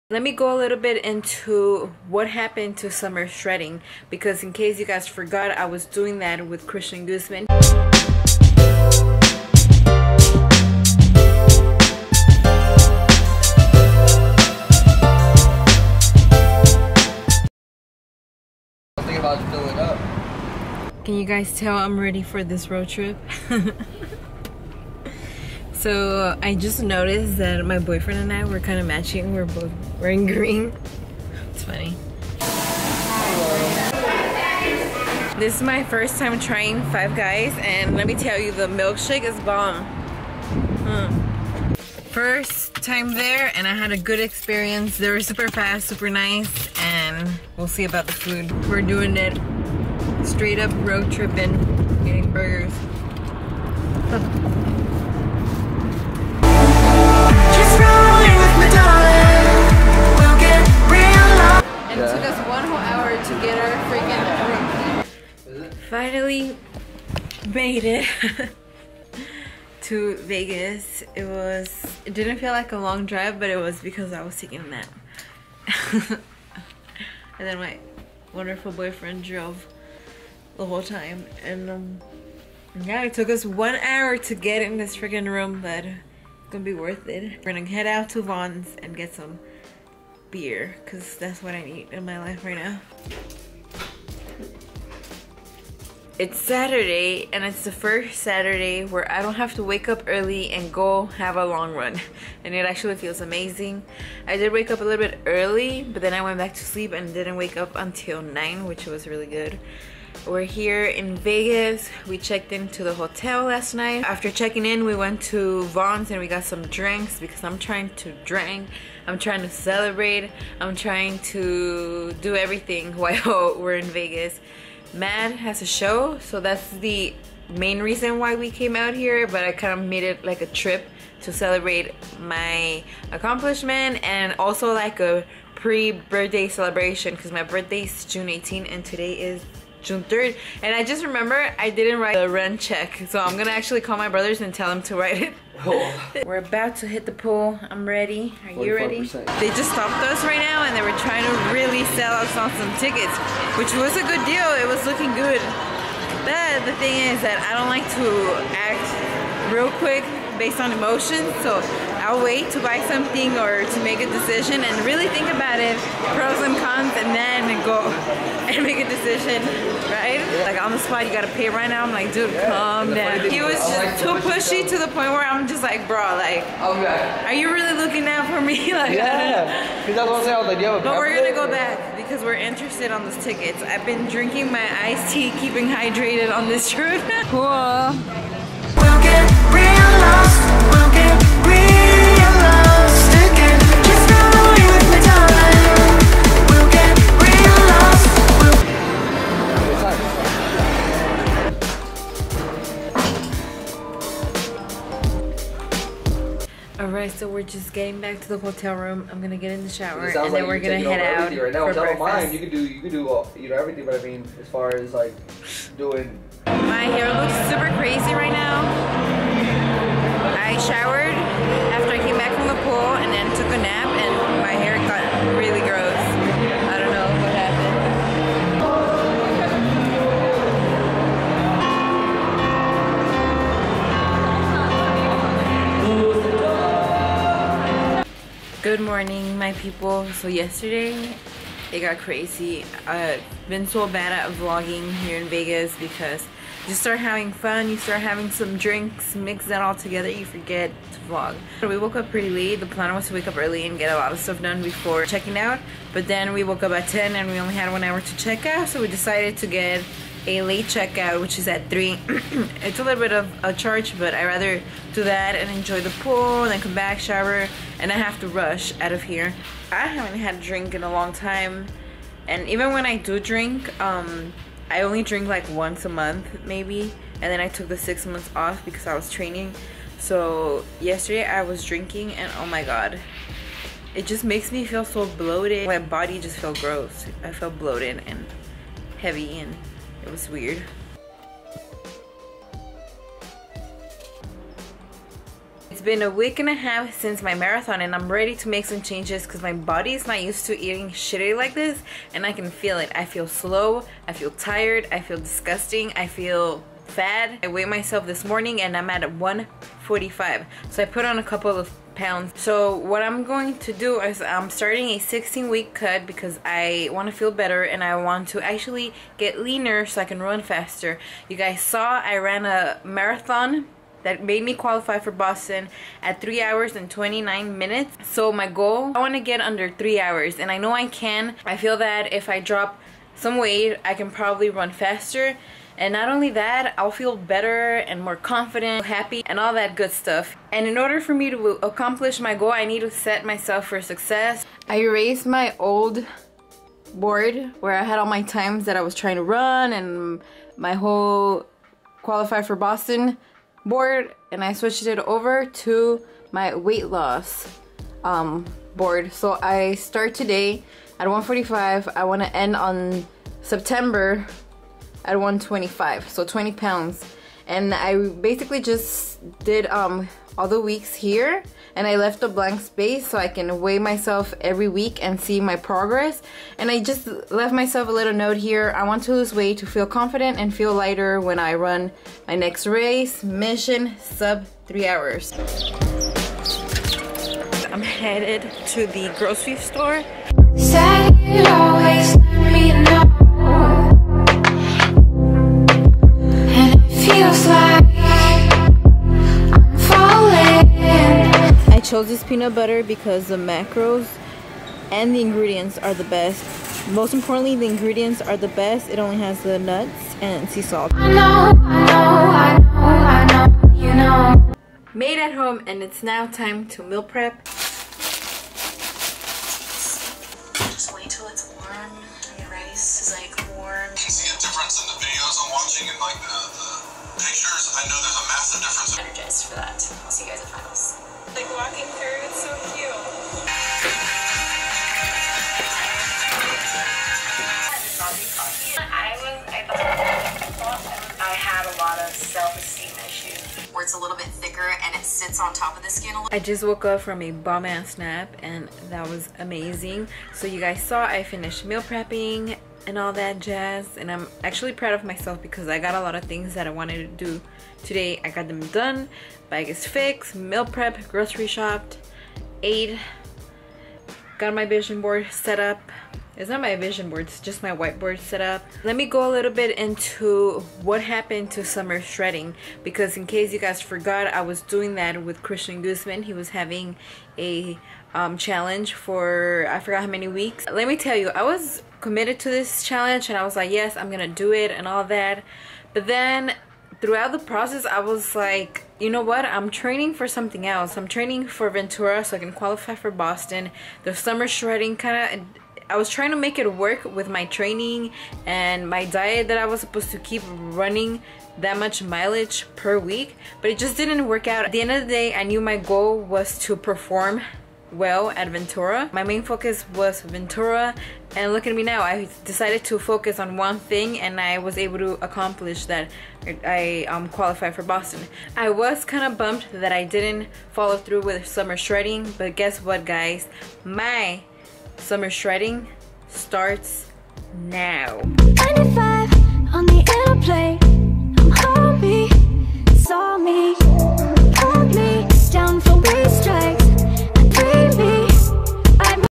What happened to Summer Shredding | Vegas | Forward Ep. 20
Boston Qualified marathon runner Jamie Jimenez takes you through her tedious lifestyle of nutrition, macro counting, running, fitness, and much more.

Camaras: Nikon D5300 & Sony RX100 4